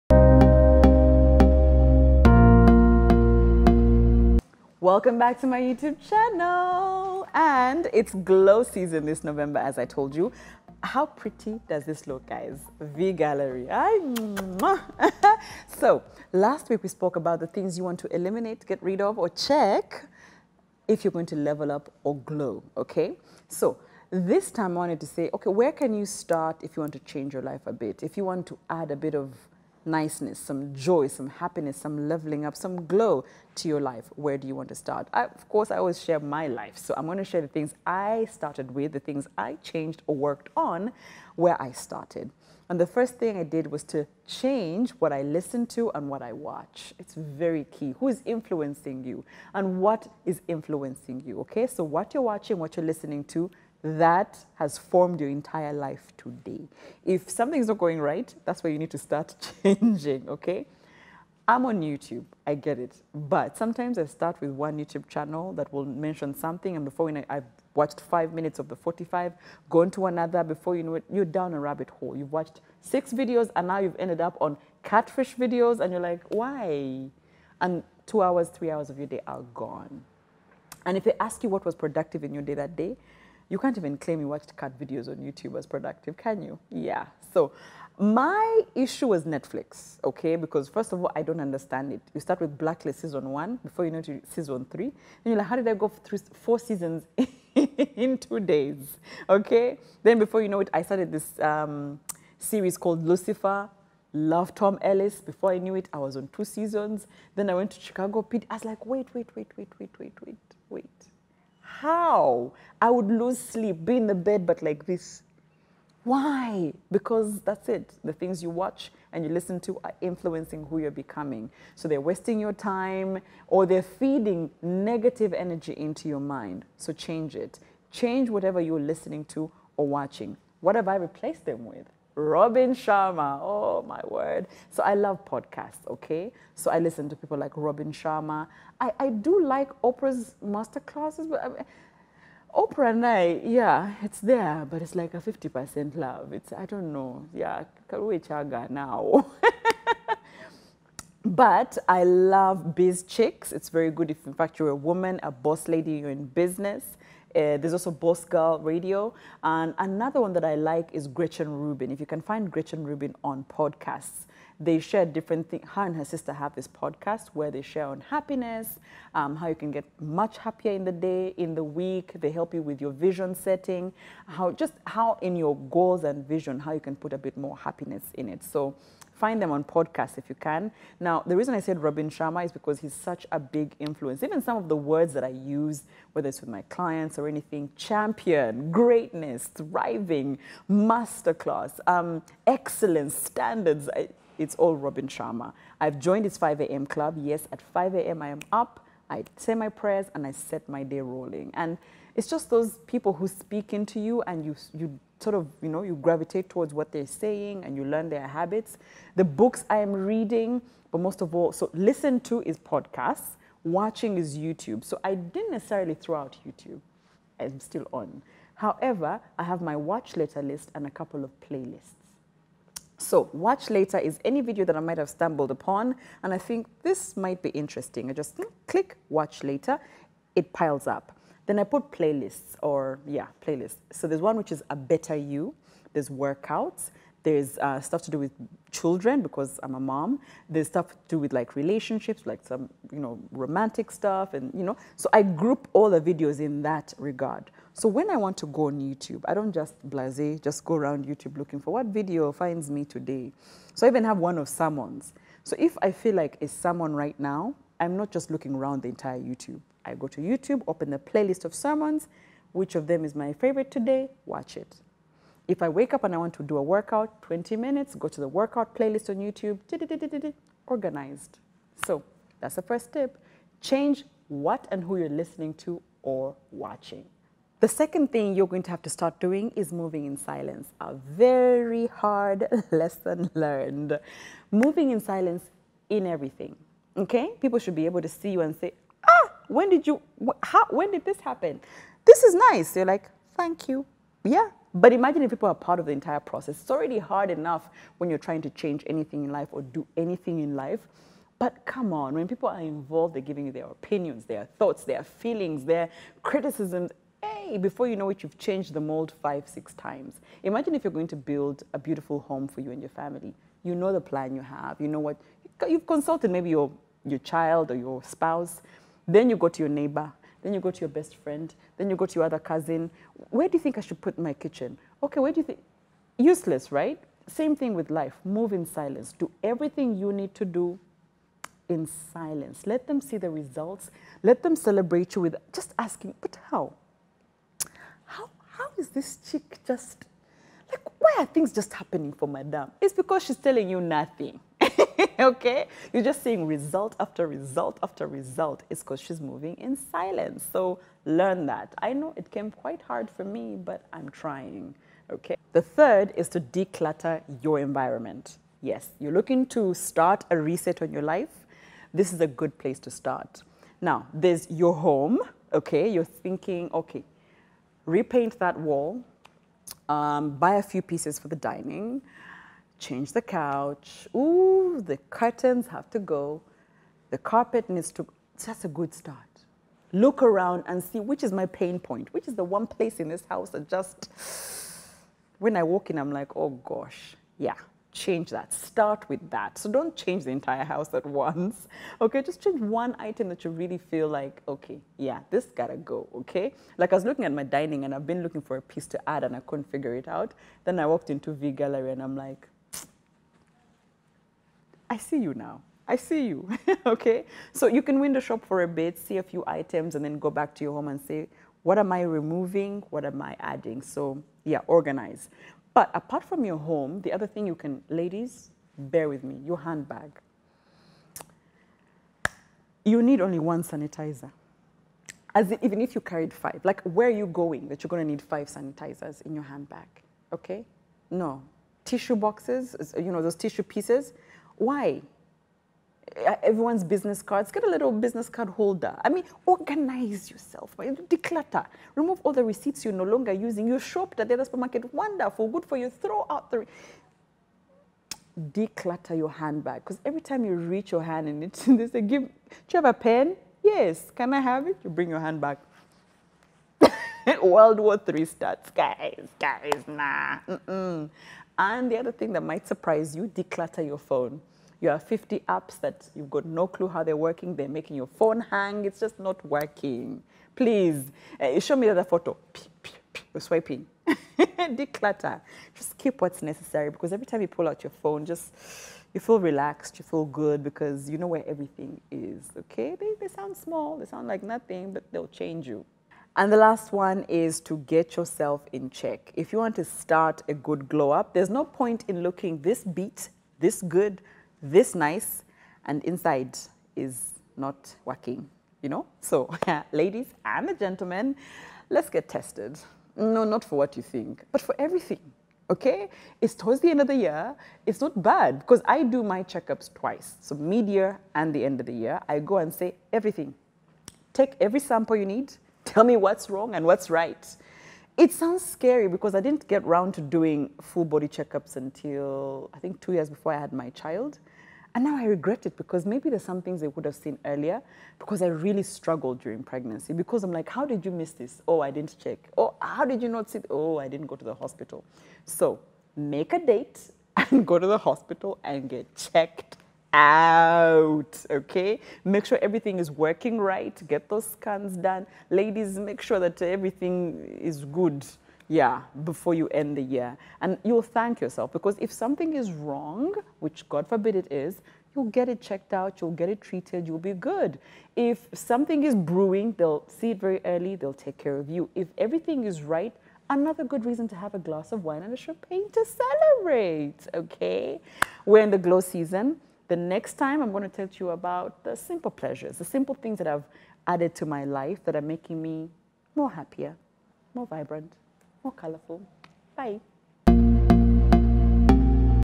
Improve your life NOW.
Last week, i shared on the habits to eliminate if you intend to live a better life.

In this vlog, i share the start, where do you begin to change your life? new habits and more.

Happy watching.